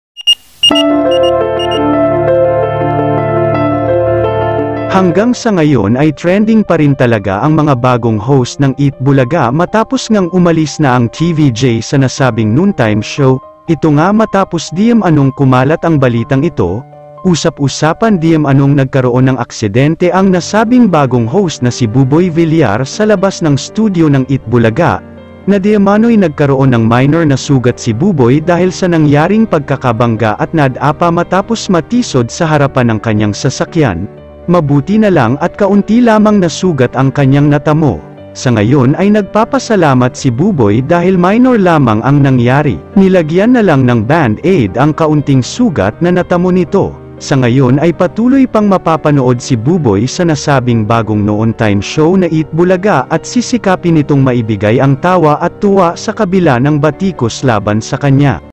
JUSKO‼️ Buboy Villar ‼️ NAAKSIDENTE sa LABAS ng studio ng Eat Bulaga!! KRITIKAL!!OMG!!
nGAYON lang!!Buboy Villar Naaksidente sa labas ng studio Ng eat Bulaga!!OMG!!